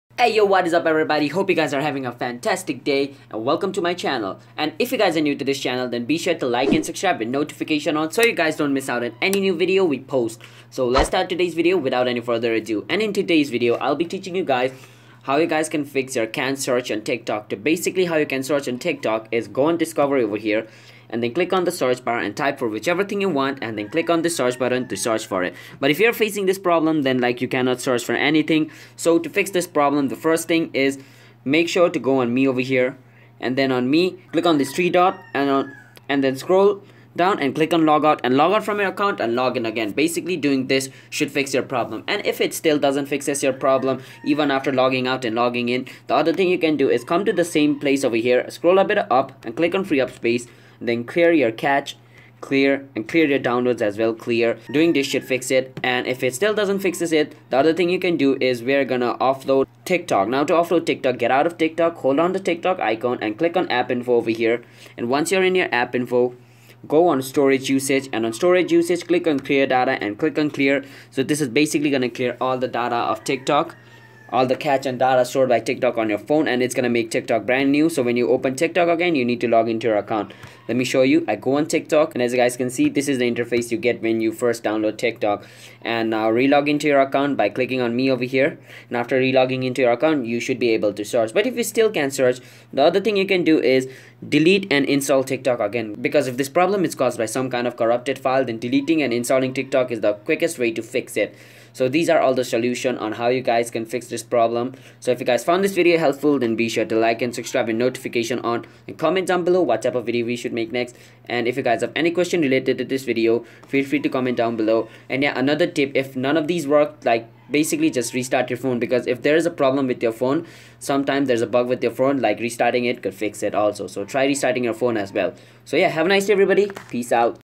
Why cant i Search On TikTok Fix!
In this video I will be showing you why you can't search on TikTok and how to fix the problem.
TikTok is one of the best social media platforms for people to share their content and get a huge following. And with that comes all sorts of problems. Like when you can't find what you're looking for in the search bar, it's probably because your phone isn't set up properly or something else is going on. So I'm here to show you some of the reasons that cause your inability to search on TikTok. It's a really easy fix which only takes about a minute or two.